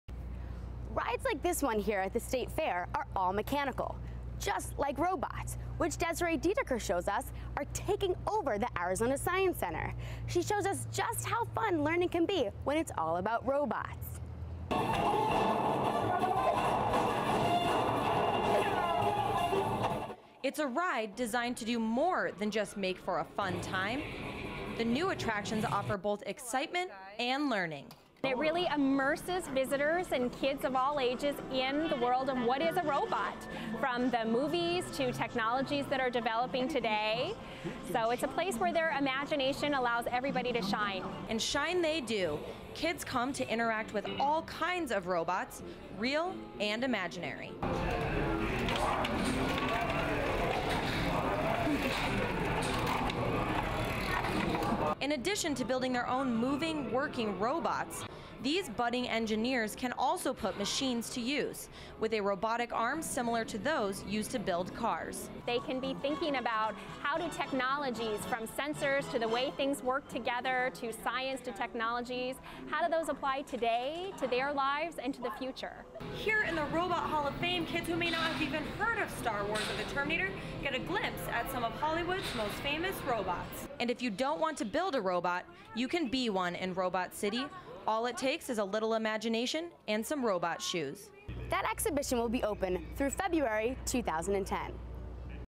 Robots - the exhibition
The Arizona Science Center has a new exhibit that may interest budding engineers. Cronkite News reporter, Desiree Dietiker, has the story.